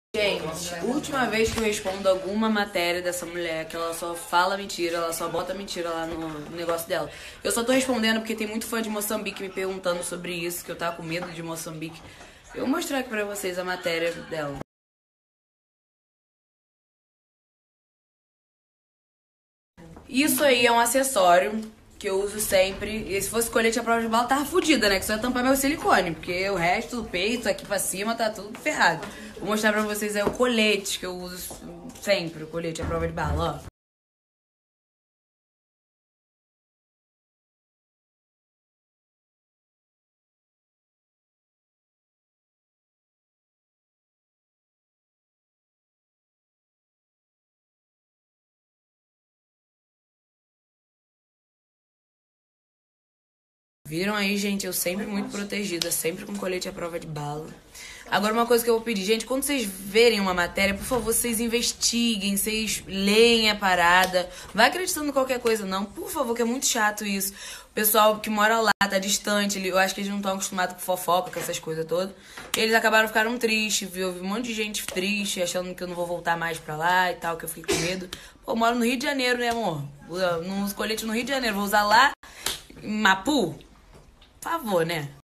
Ludmilla esclareceu uma notícia FALSA sobre ter utilizado colete à prova de balas em seus show
Ludmilla através do IG Story esclareceu uma notícia FALSA sobre ter utilizado colete à prova de balas em seus shows em Moçambique 🇲🇿
A peça trata-se de um acessório de moda sem intenção de proteção.